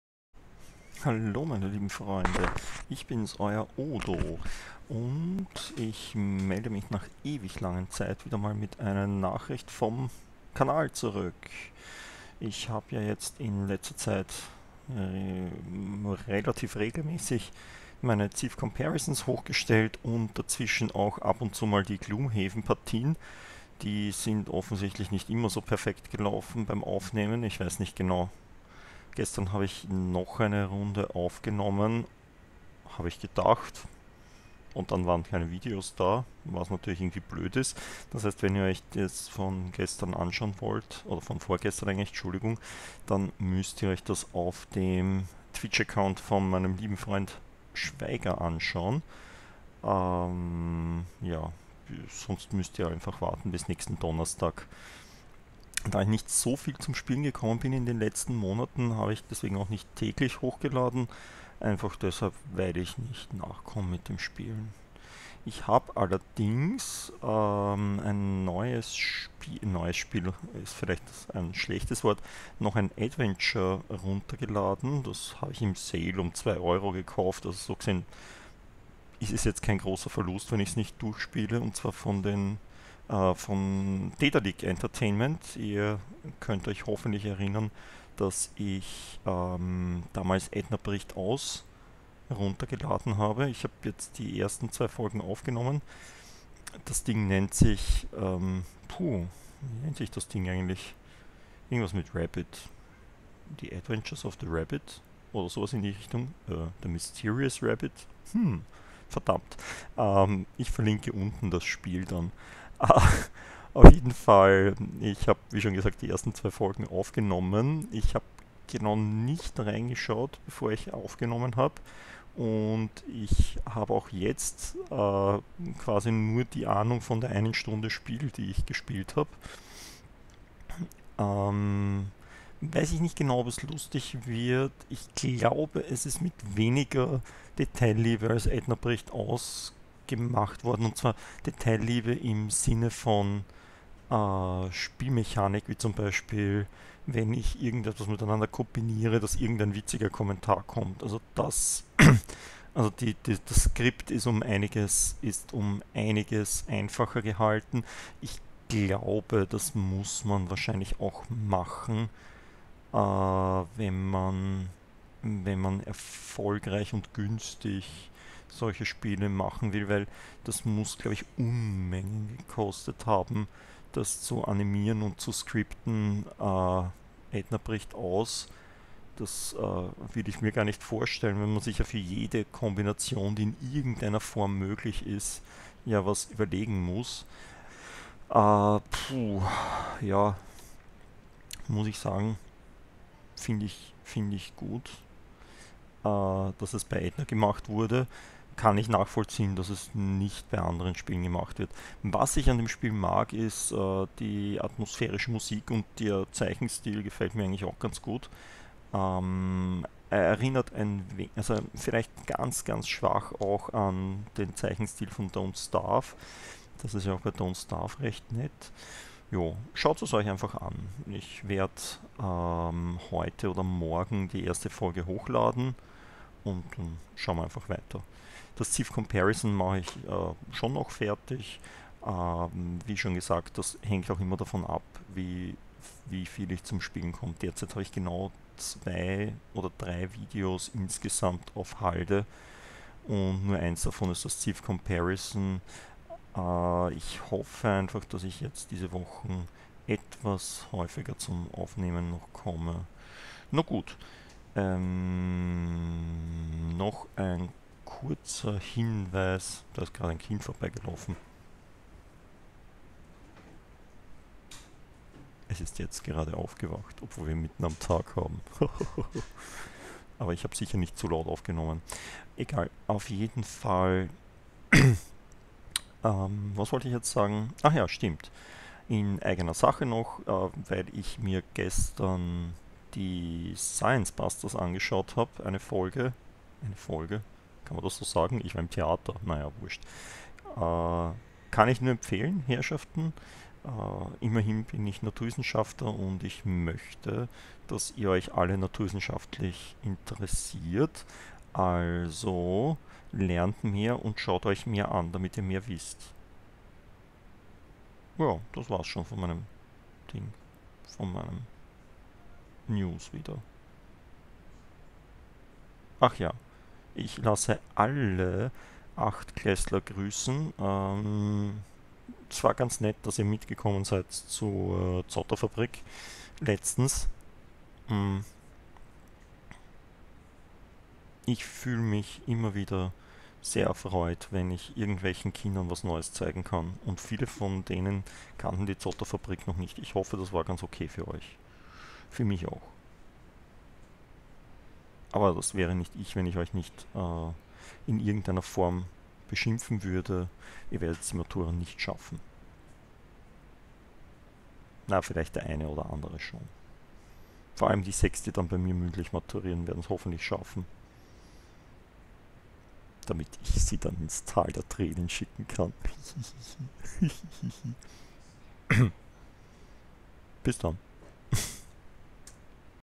Nachrichten vom Kanal 08
Ein neues Lets Play wird von mir gestartet. Nennt sich "The Night of the Rabbit" und kann unter anderem
gekauft werden.